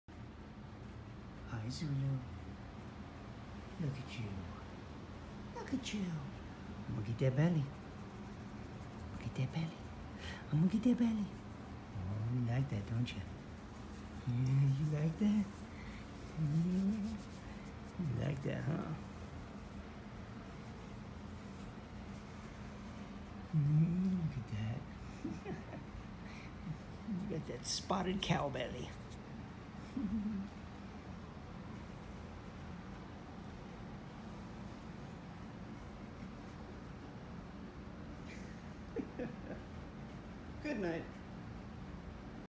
Basenji Zulu Gets Another Belly Rub | Dog with a spotted cow belly!
Some say Zulu dog has a cow belly... what do you say?